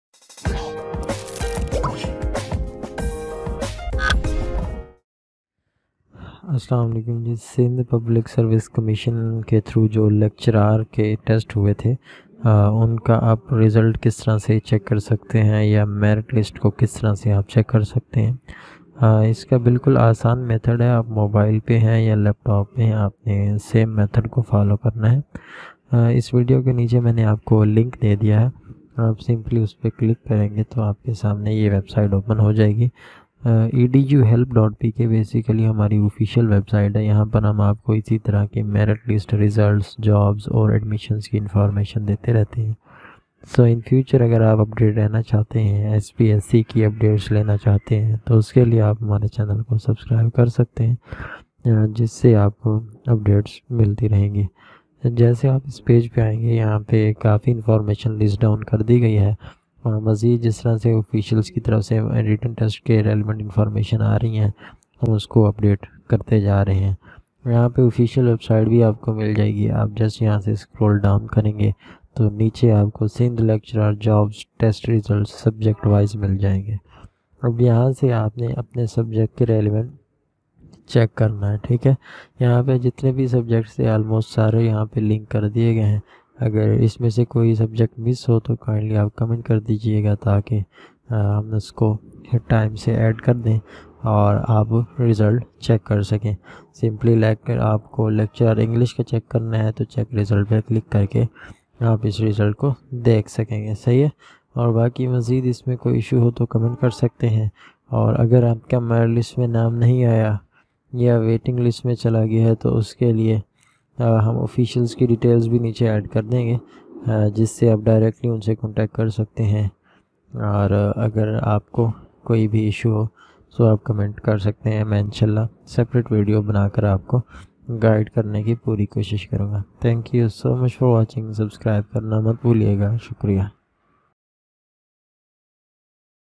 SPSC Lecturer Result 2021 Merit List Subject Wise
SPSC Lecturer Result 2021 Merit List, interviews list and list of selected and rejected candidates check online. Check Subject wise result of  Bio-Chemistry, Botany, Chemistry, Computer Science, Mathematics, Microbiology, Physics, Zoology, Commerce, Economics, Geography, Statics, Education, English, Muslim History, Islamic Studies, Pakistan Studies, and also Sindhi. Sindh Lecturer Jobs SPSC Interview Date & Final Merit List Check Online. How to check Lecturer Written Test Result 2021 SPSC College Education?. Click on the link to check SPSC Written Result 2021 Lecturer Jobs.

Related Information
SPSC LecturerTestDate2021Result